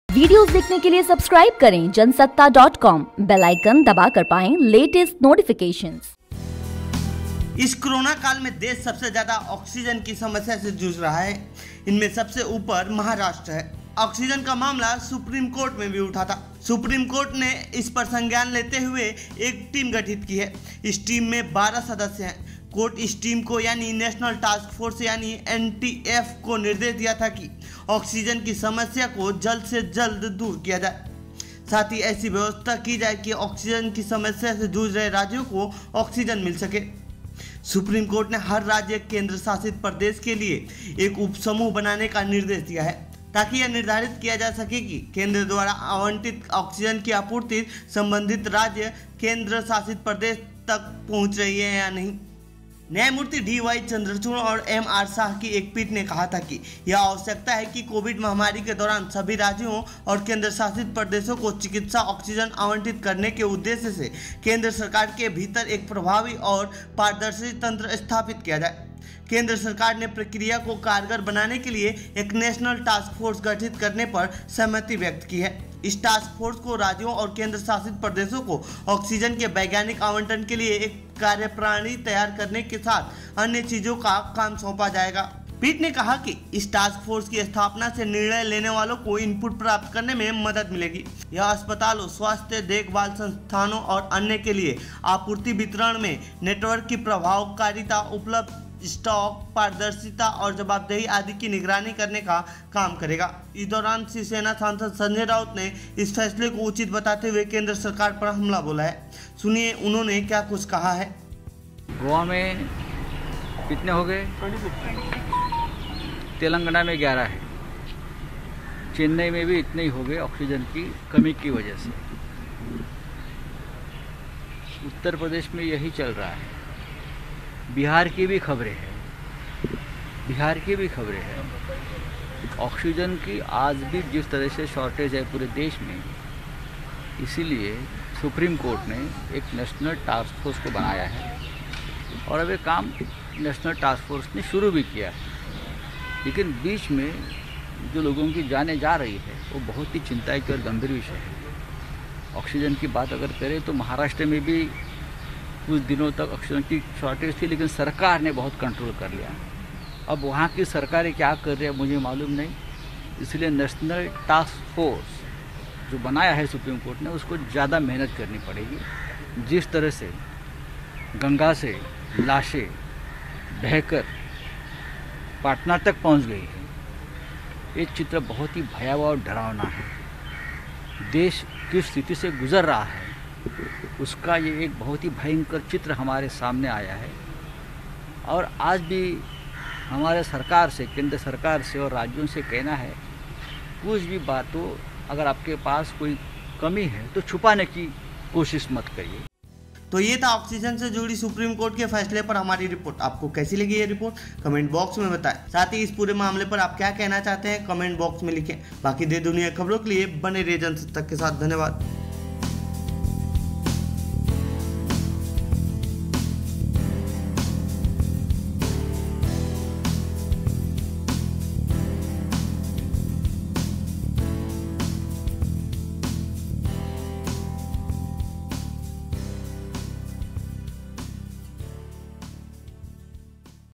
सुप्रीम कोर्ट के फैसले को शिवसेना सही ठहराया, बोले राउत- NTF को करनी होगी कड़ी मेहनत
इस कोरोना काल में देश सबसे ज्यादा ऑक्सीजन की समस्या जुझ रहा है. इनमें सबसे उपर महाराष्ट्र है. ऑक्सीजन को लेकर राज्य सरकारे सुप्रीम कोर्ट गई थी. सुप्रीम कोर्ट ने इस पर संज्ञान लेते हुए. एक टीम गठित की है. इस टीम में 12 सदस्य हैं. कोर्ट इस टीम को यानी नेशनल टास्क फोर्स यानी एनटीएफ को निर्देश दिया था. अब इस पर शिवसेना ने अपना रुख साफ किया है. सुनिये शिवसेना नेता ने क्या कुछ कहा है।